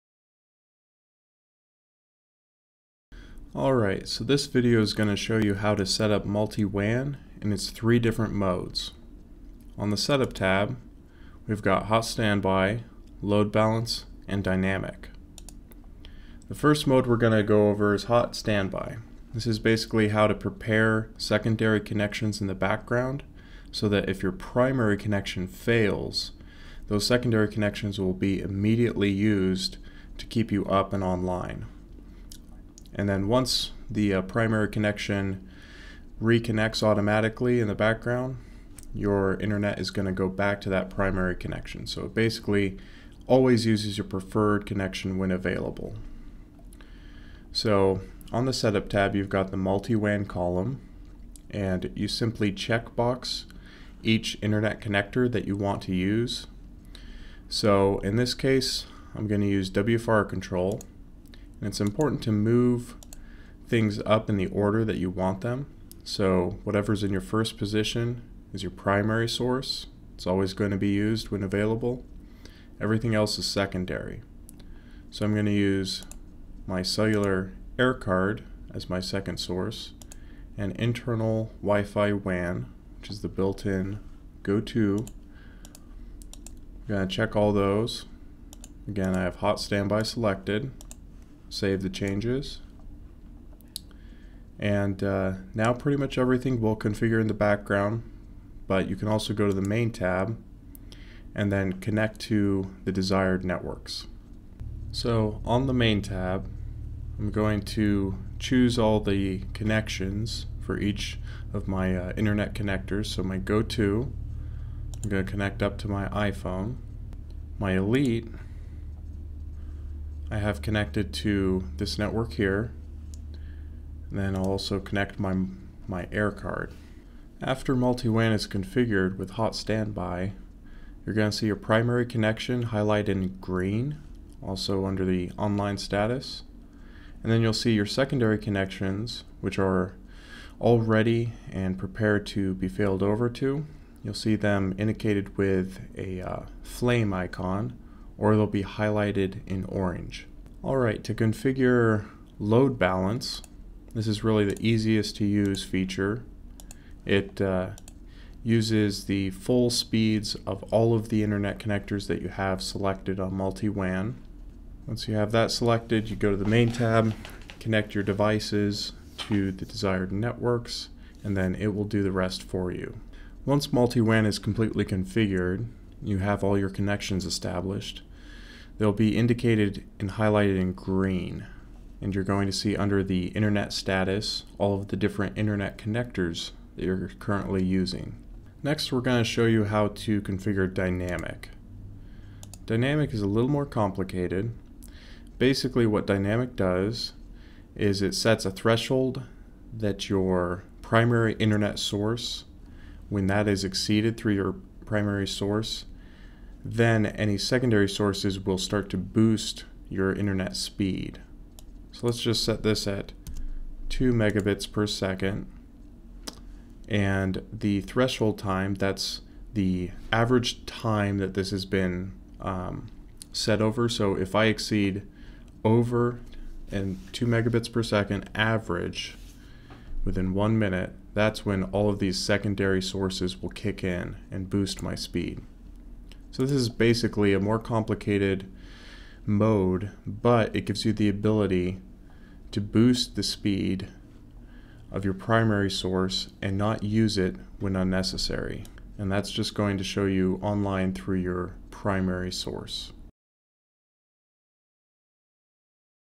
WiFiRanger setting up Multi-WAN
WiFiRanger how-to video on configuring multiple internet sources to work in conjunction.